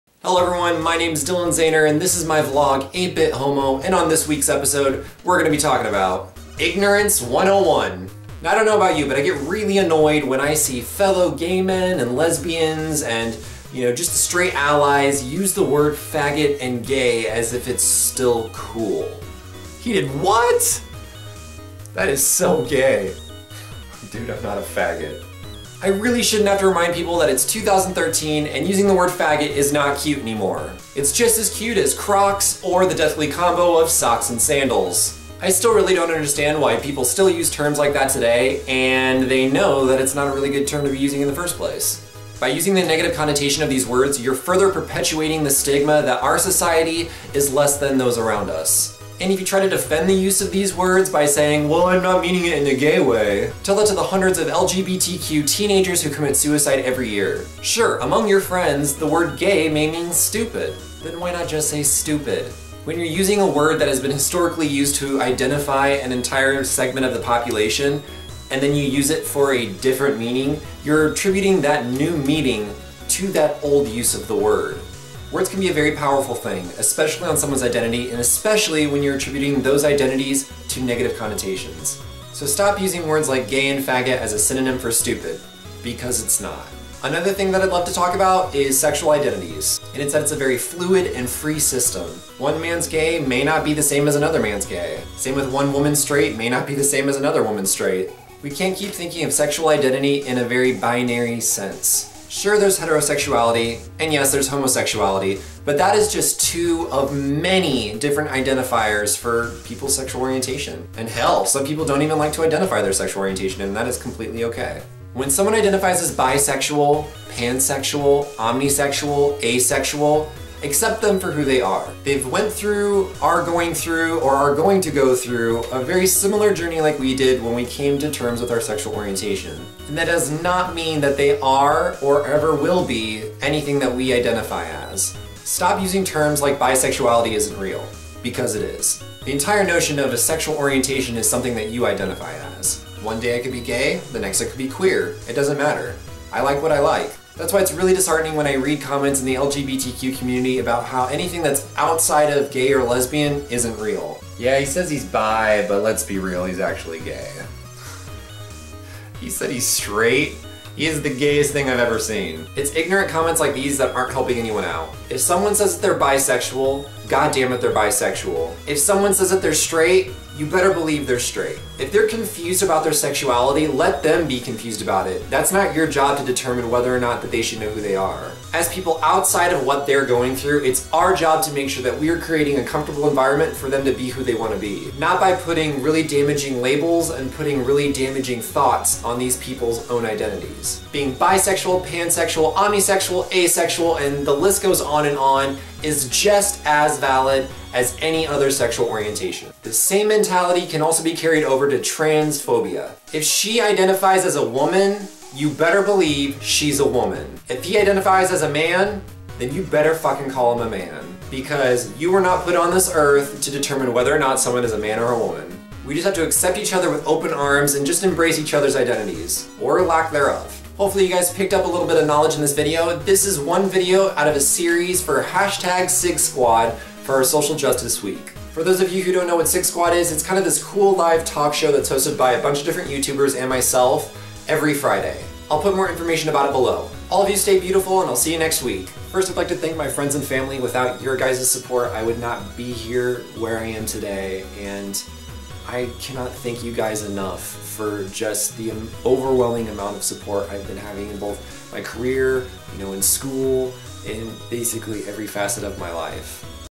8-Bit Homo: Ignorance 101
Time to talk about the use of words like "faggot" and "gay", sexual identities, and transphobia in my video for SigSquad Social Justice Week!